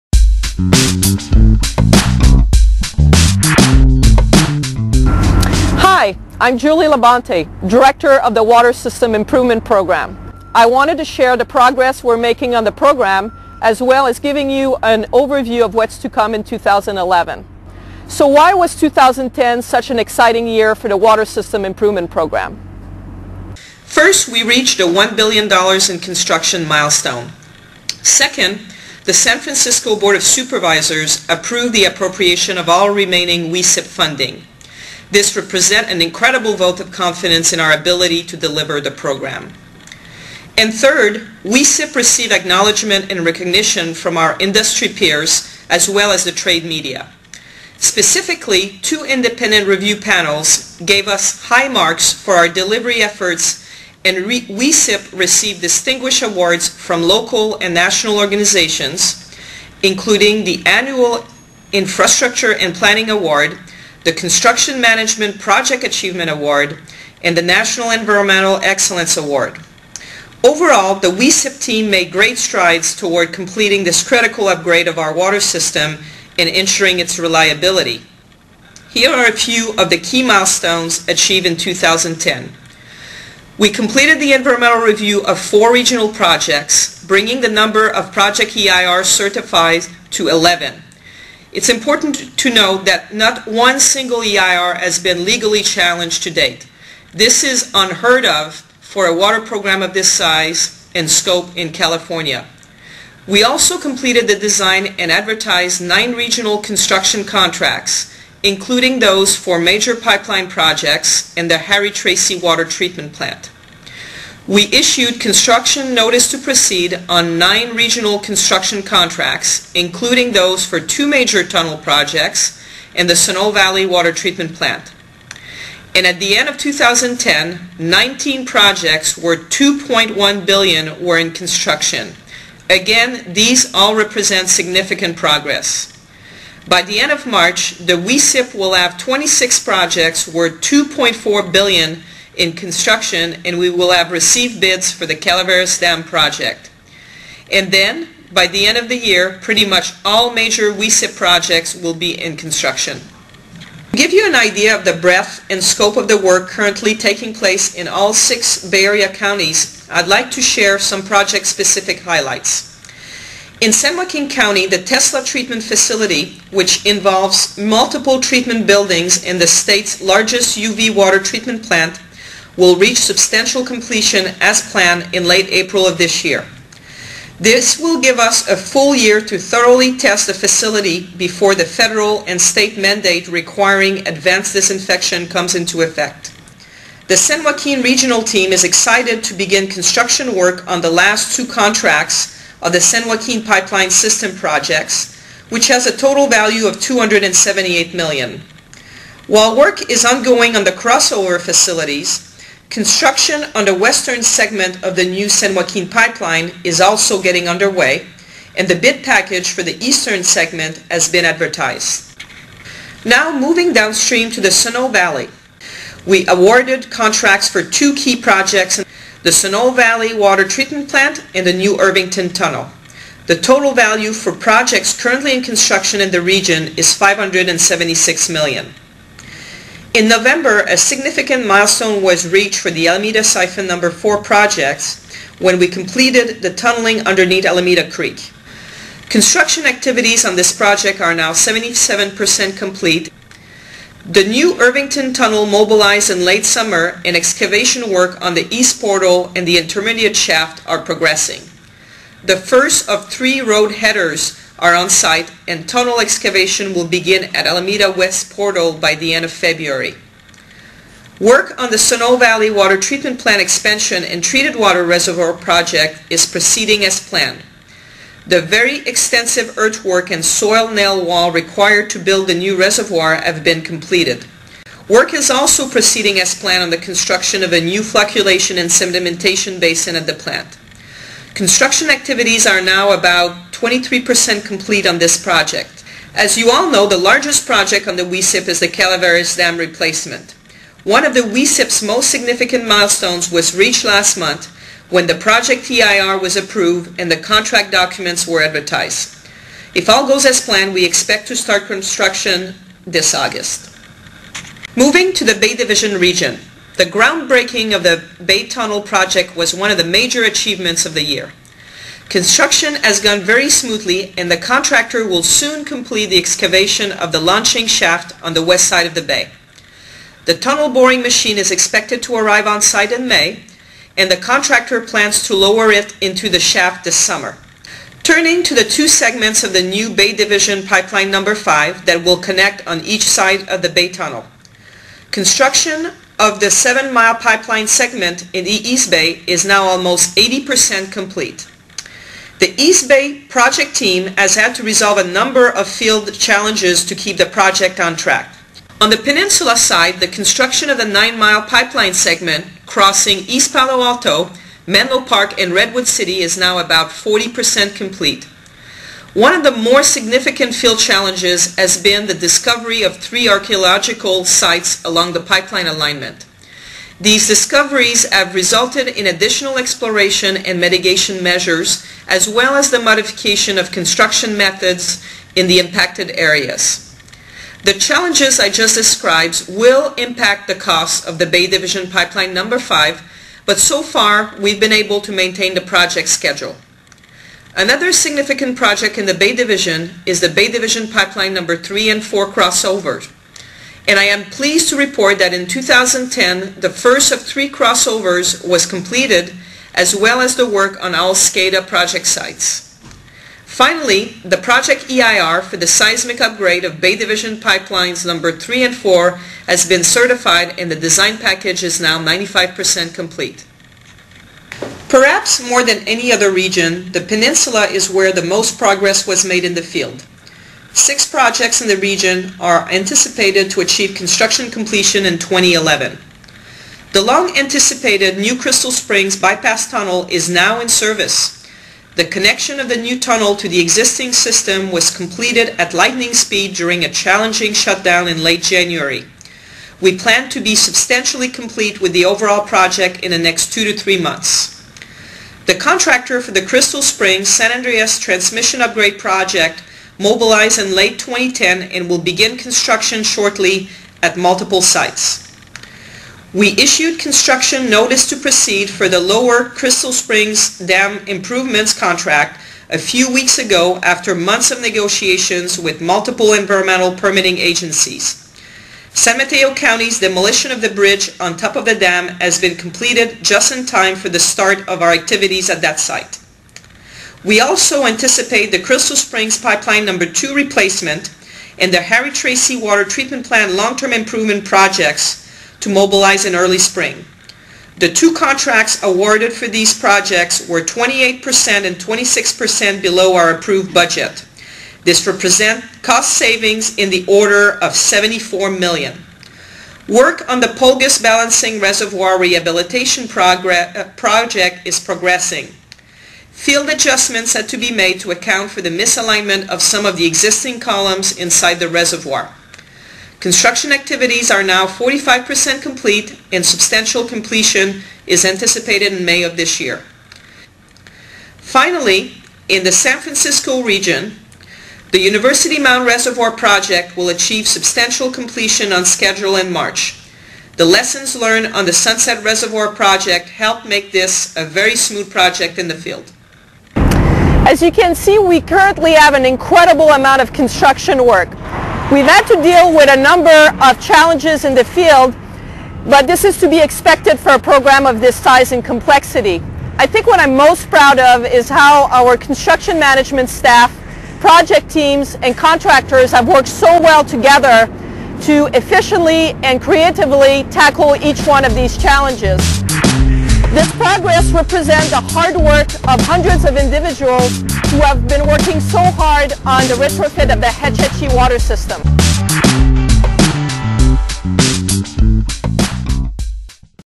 What's Up With WSIP?
Julie Labonte, Water System Improvement Program (SWIP) Director provides an update on SFPUC's WSIP for 2010 and what's to come in 2011.